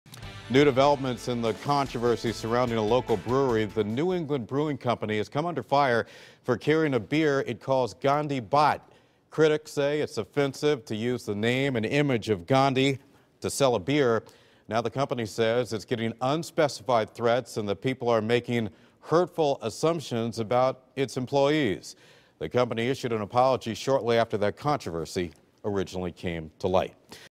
Brewery seeks to end uproar over Gandhi beer name, label
A Connecticut brewery that drew condemnation in the U.S. and India for its beer that carries the name and likeness of Mohandas Gandhi says it’s received threats and is seeking a way to end the uproar.